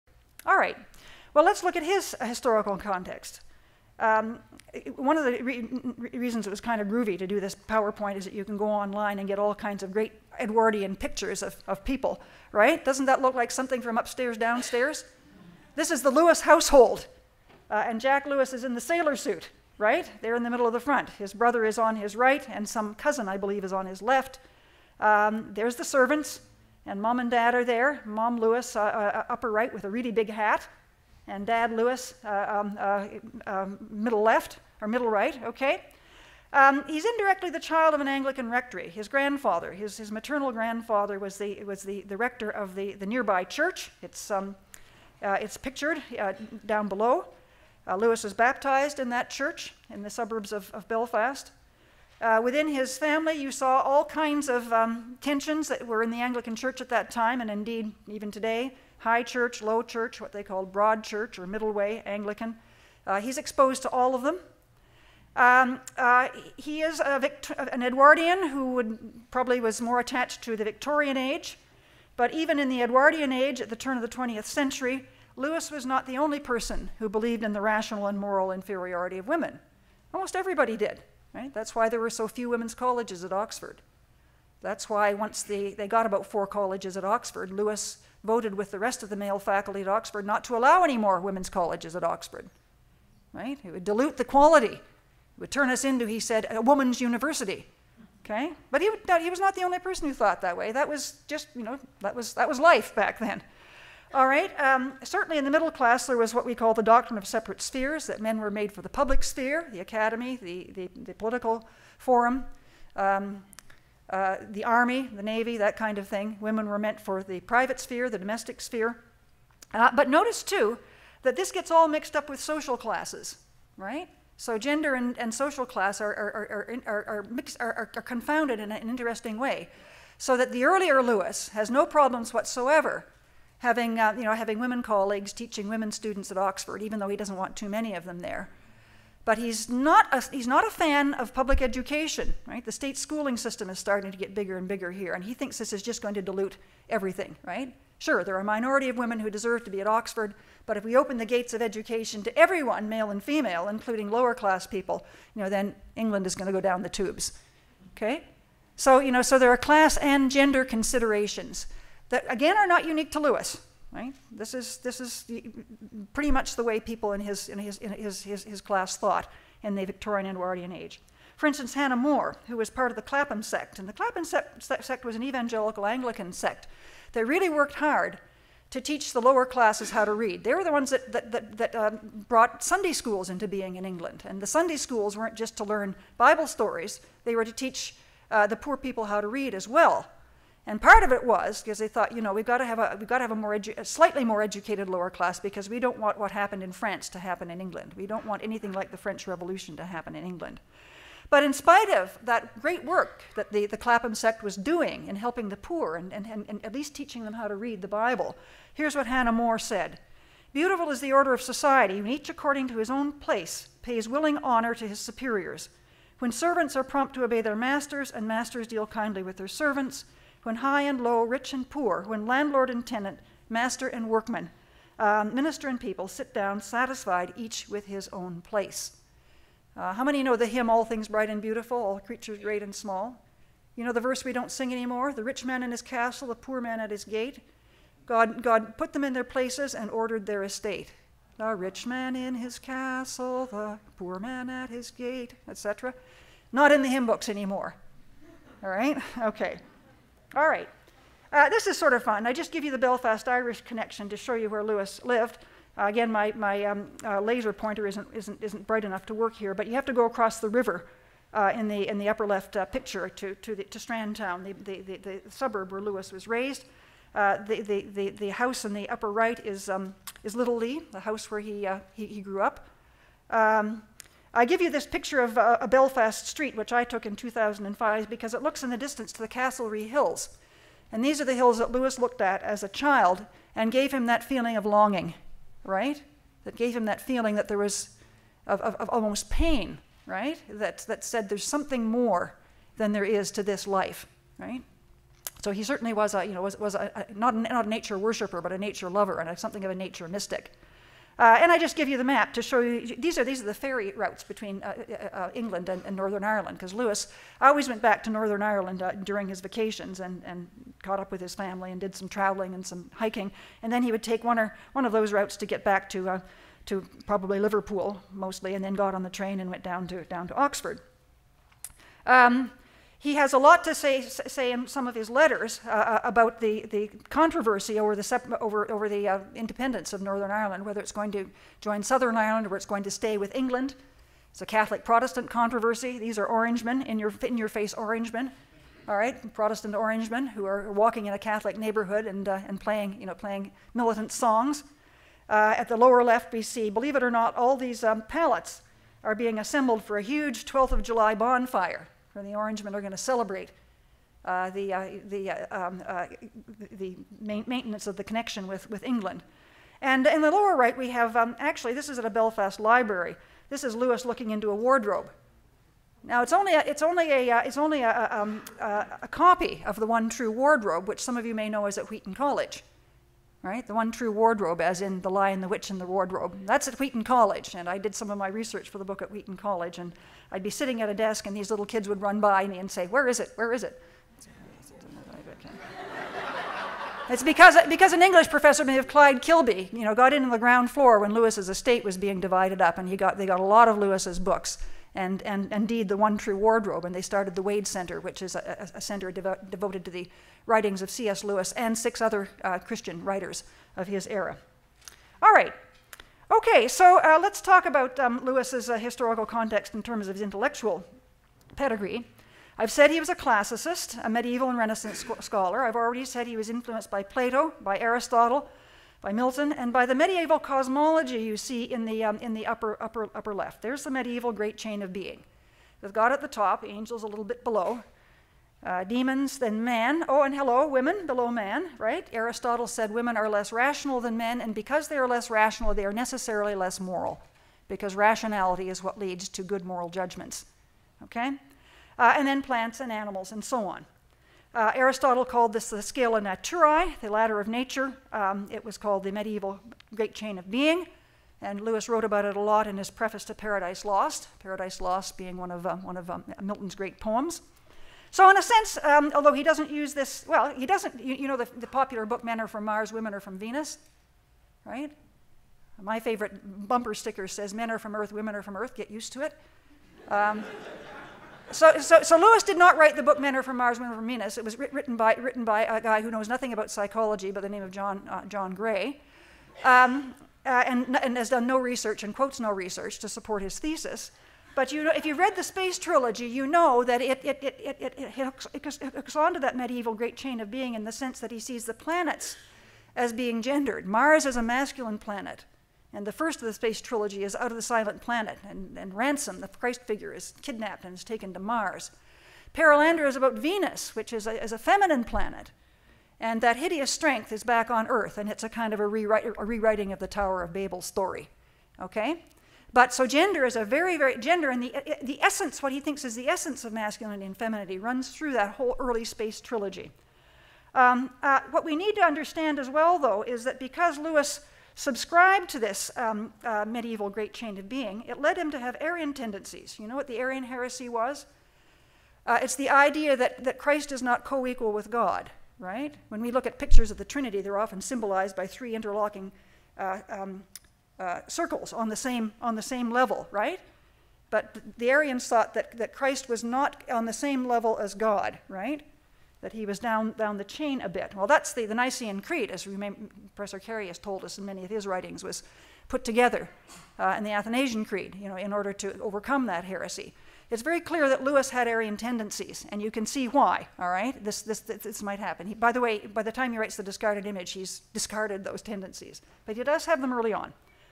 Mary Stewart Van Leeuwen - Part 3
October 8: Homecoming Weekend. Mary Stewart Van Leeuwen, EU Professor of Psychology. "A Preference for People: How C.S. Lewis changed his thinking about gender". Mary will discuss her new book A Sword Between the Sexes: C.S. Lewis and the Gender Debates.